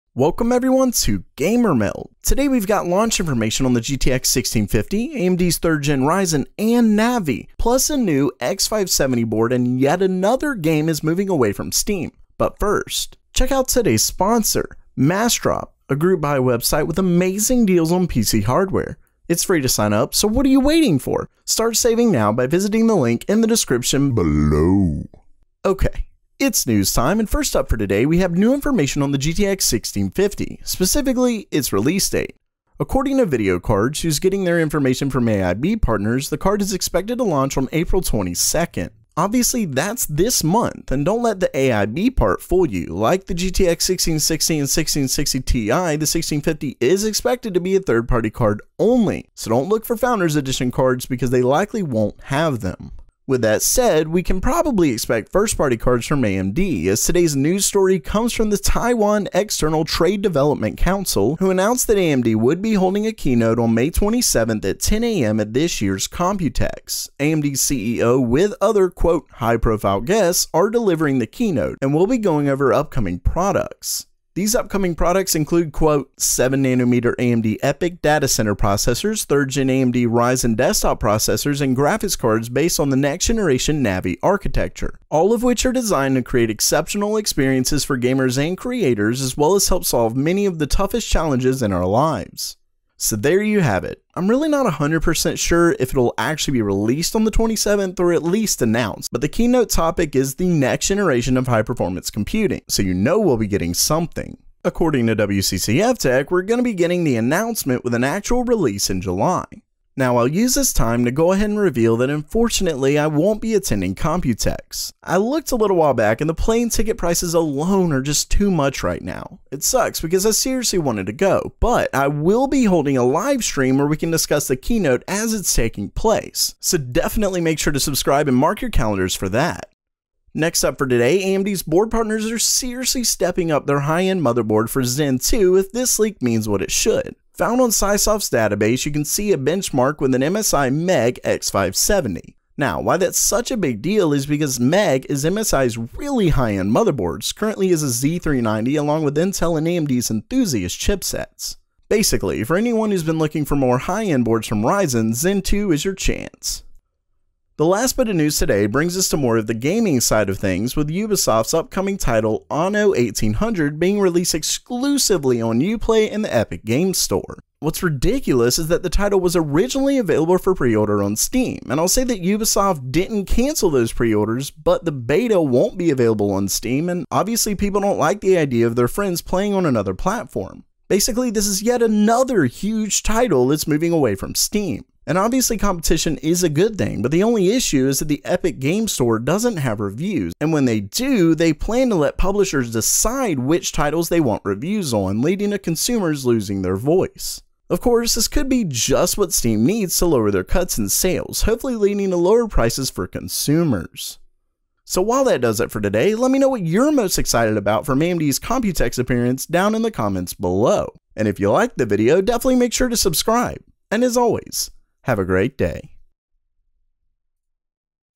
OFFICIAL 3rd Gen Ryzen & Navi Reveal!
AMD's 3rd generation Ryzen CPUs and Navi have an announcement date, GTX 1650 gets a launch date, X570 MOBO and Steam isn't doing well.